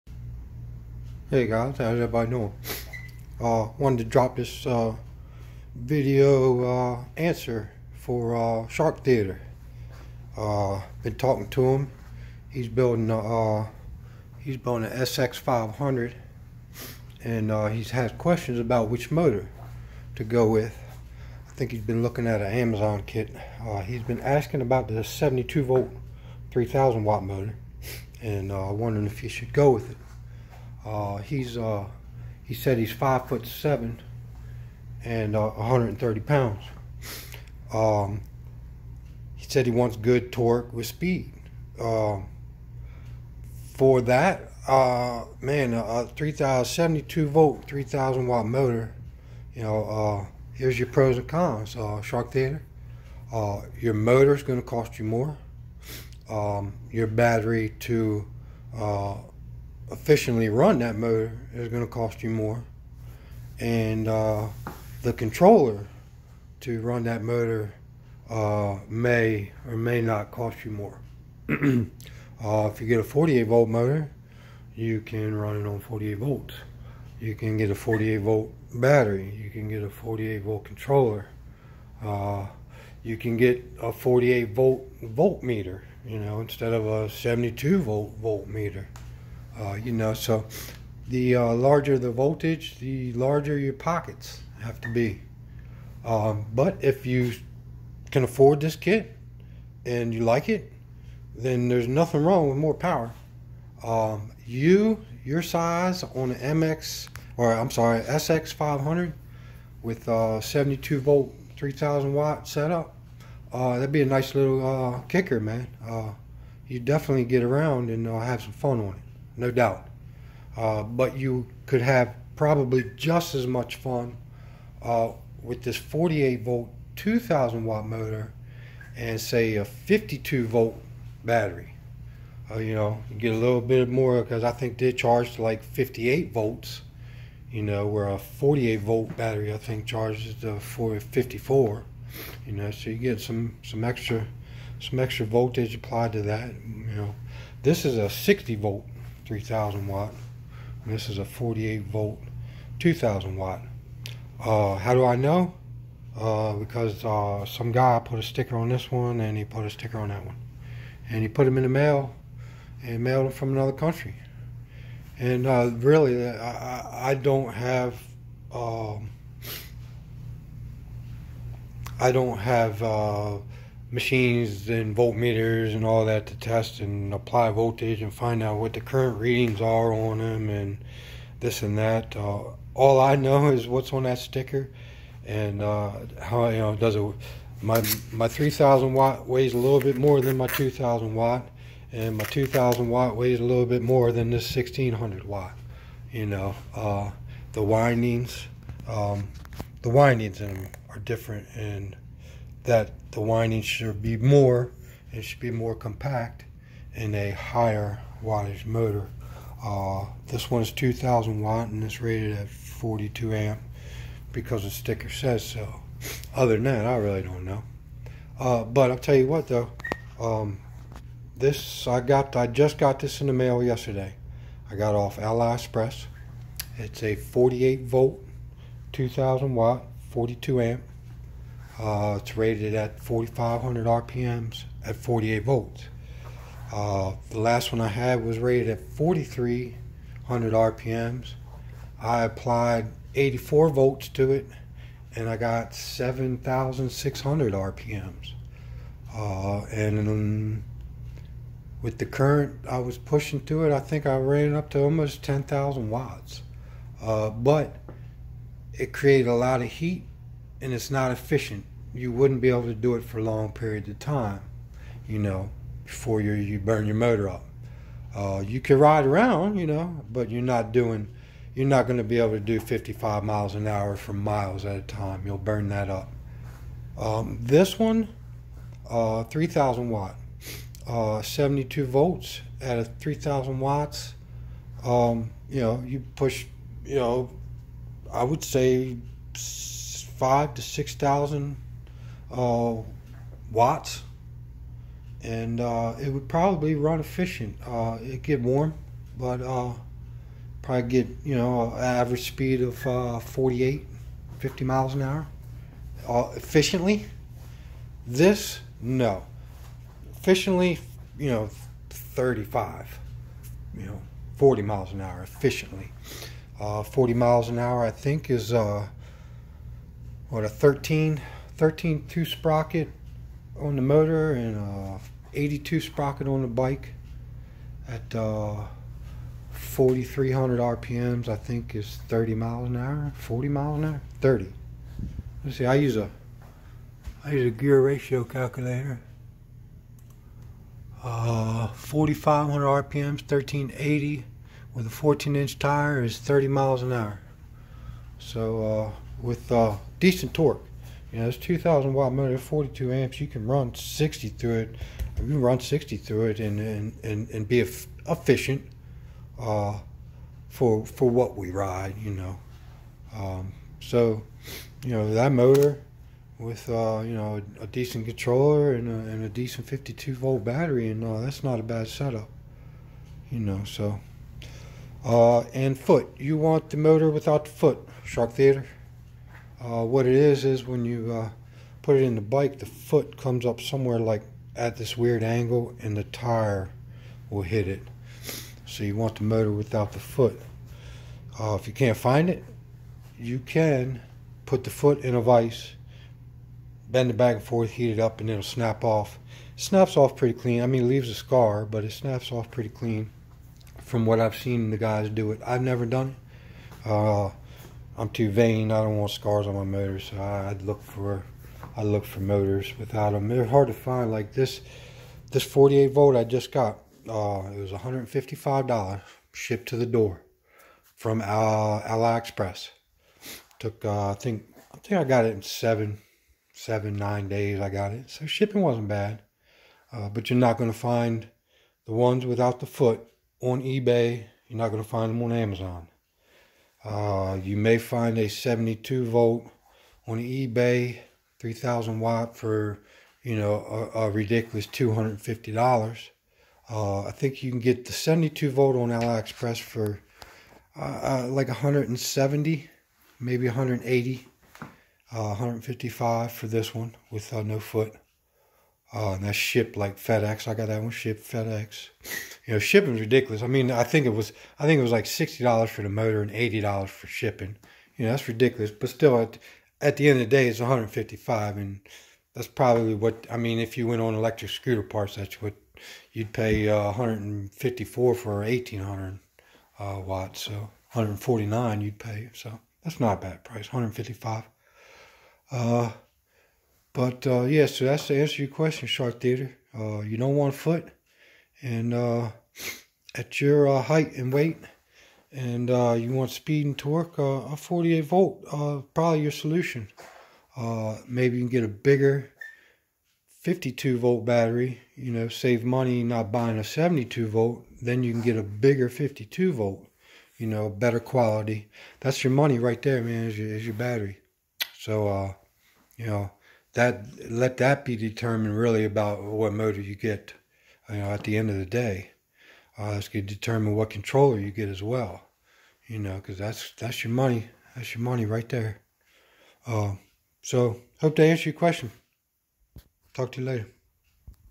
Razor RSF650 - Motor Talk … 2kw or 3kw?
Video response to a viewers question about motor current and what set-up is best for his size & riding conditions.